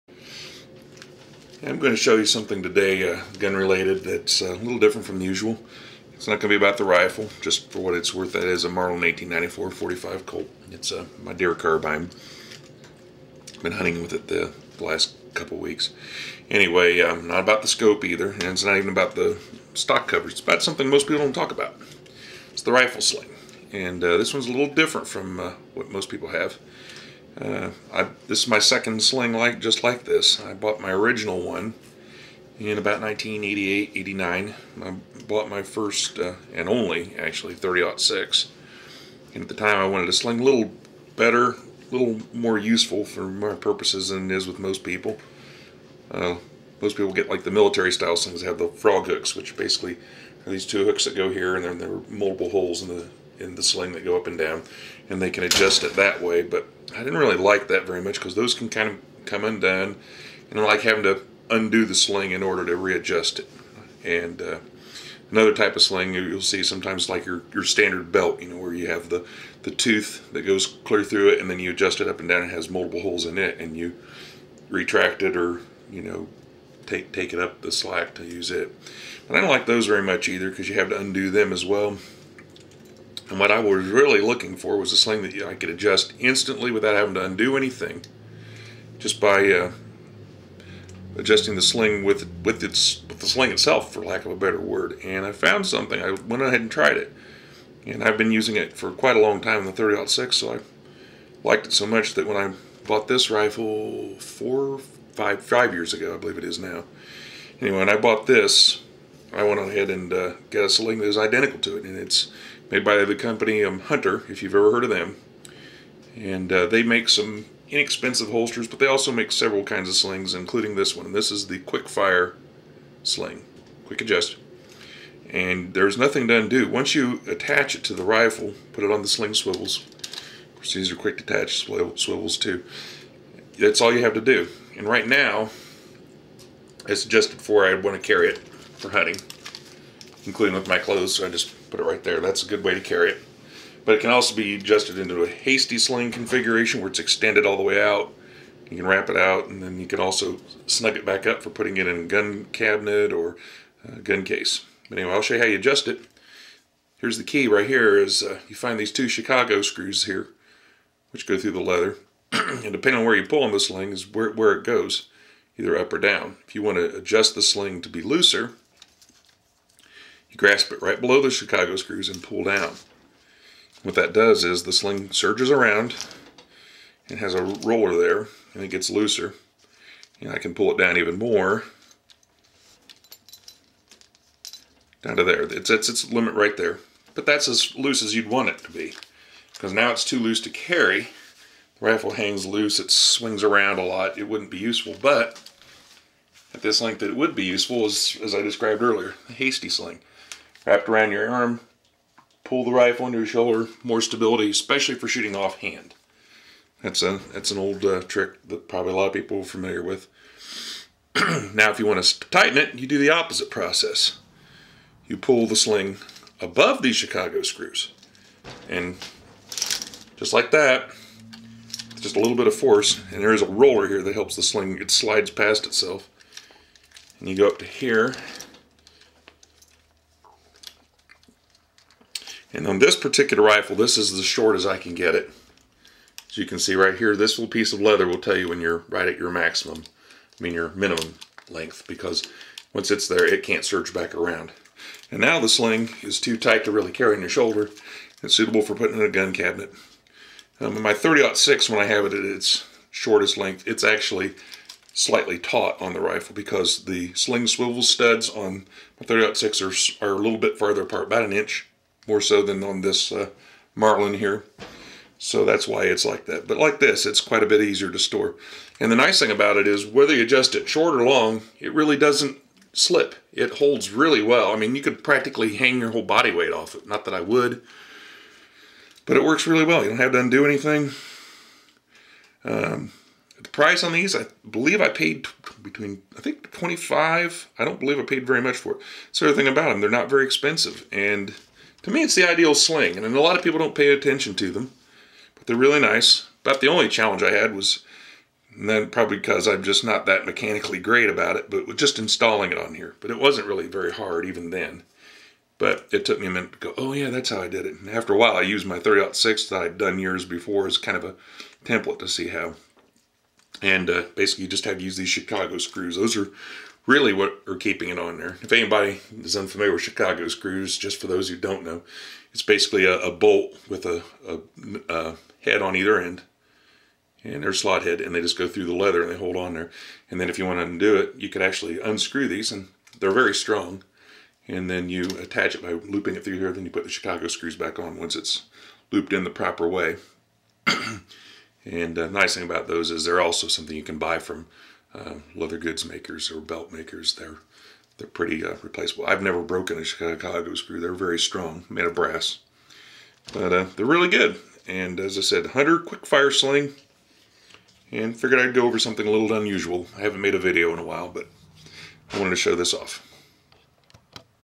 "Quick Fire" Rifle Sling review
My short review and demonstration of the Hunter Quick Fire sling.
As I explain in the video itself, this is my second of the exact same sling, so it should be obvious that I really like it.
This is not a paid advertisement of any sort, only an opportunity to express my thoughts and experience (over twenty-five years as of 2015!) with this excellent product.

For the benefit of anyone wanting to get some good illustrations for how to assemble this sling: